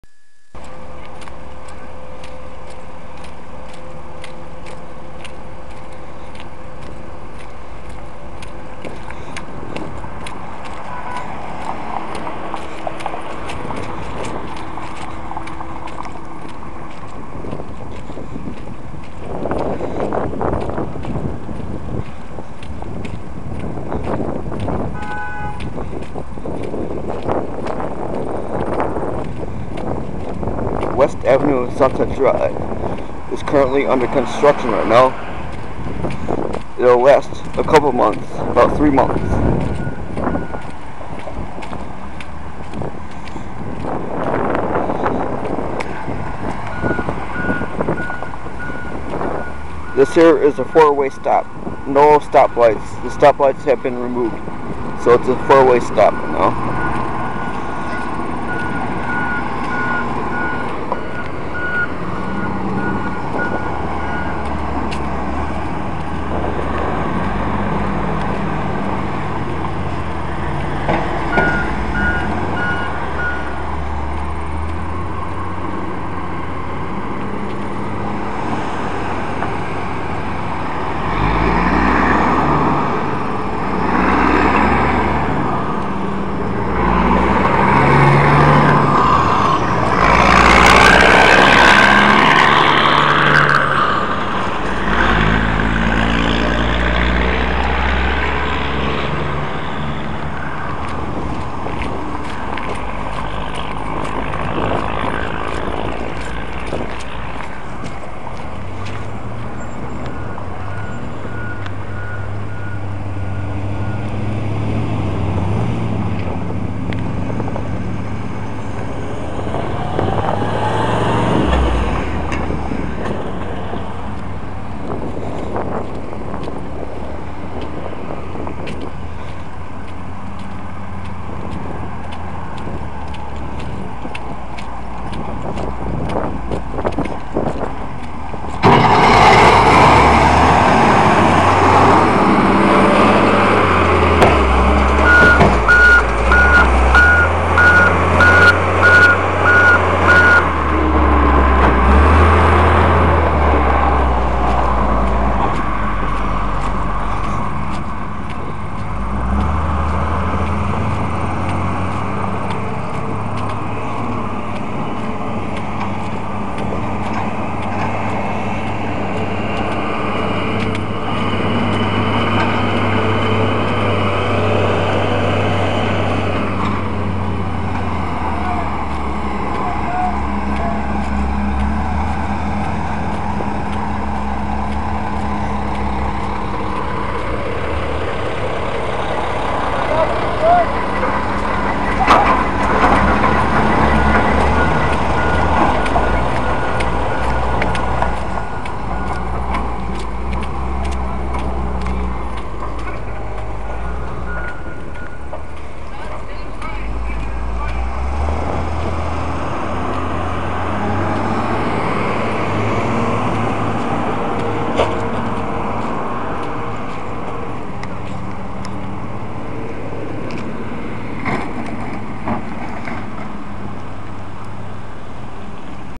West Avenue Waukesha
ROAD WORK IS NOW COMPLETE
this is SW West avenue and W Sunset drive in Waukesha. this spot is currently under construction as of 8/12/2011.

here you see construction barrels, and torn up roads and motorists passing by.

if driving down this way, drive carefully, and beware of possible street closers, detours and other delays due to the road construction. it's suppose to last up to three to four months.

you'll also see McDonald's on West Ave., part of it's parking lot and entrance / exit is blocked off. only one side is accessible.

PS. i love McDonald's fries and Southern Chicken Sandwich.